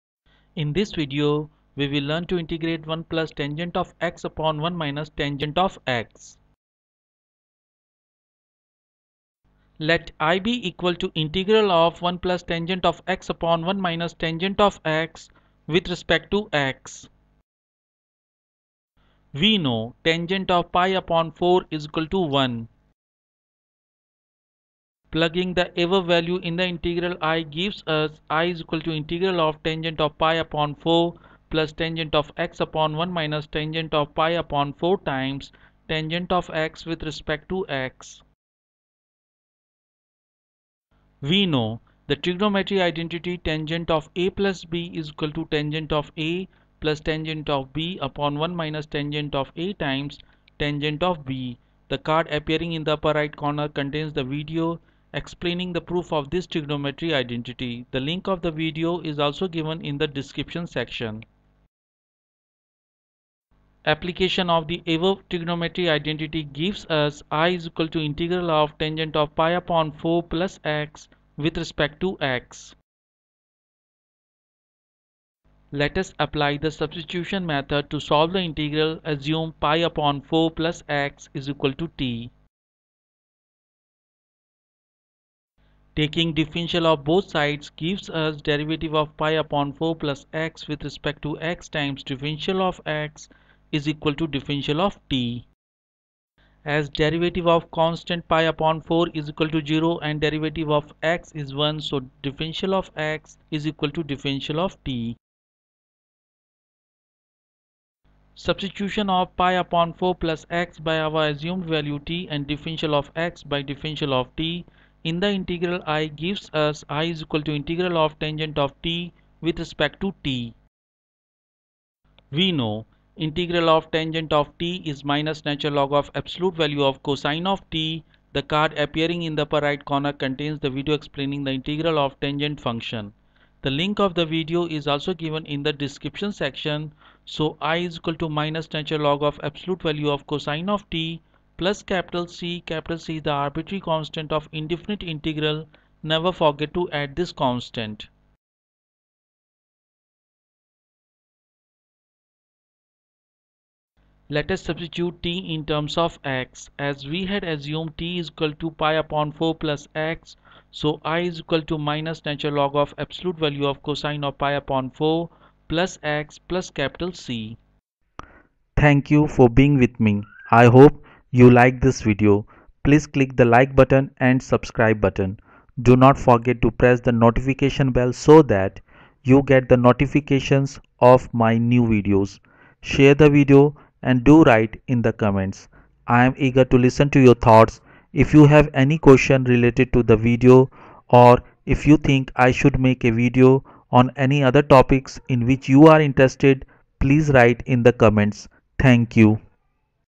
Integrate (1+tan x)/(1-tan x)
In this video we will learn to integrate (1+tan x)/(1-tan x) w.r.t. x.

The link of the video explaining the proof identity tan(A+B)=(tan A + tan B)/(1 - tan A tan B) is given below:

Other topics for the video are:
Integral of (1+tan x)/(1-tan x)
Antiderivative of (1+tan x)/(1-tan x)
Integration of (1+tan x)/(1-tan x)